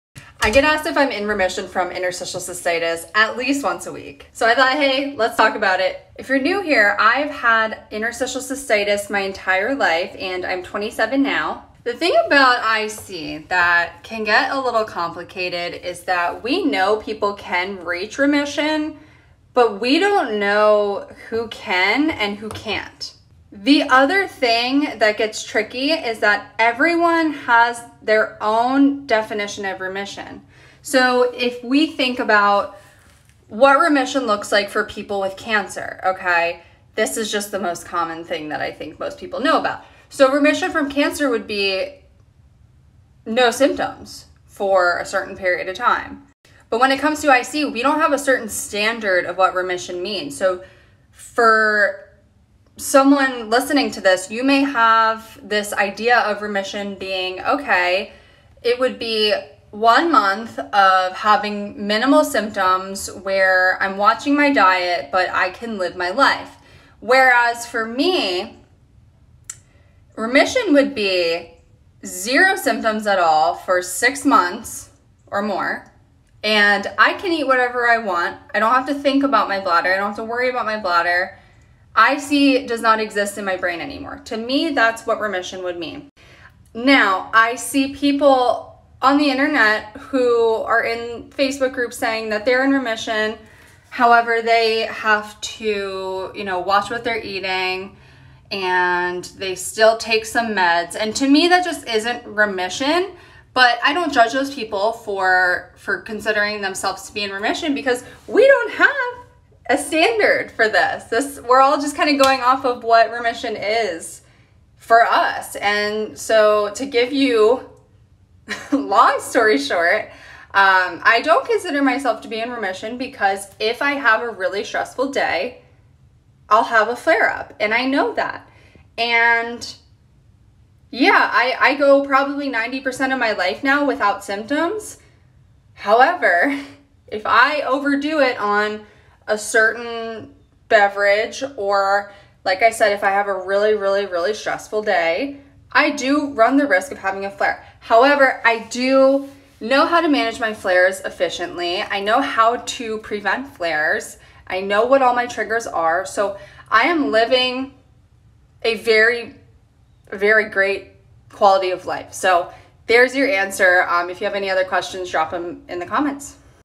Am I in remission from interstitial cystitis?
In this video I talk about what remission means to me and if I consider myself to be in remission from interstitial cystitis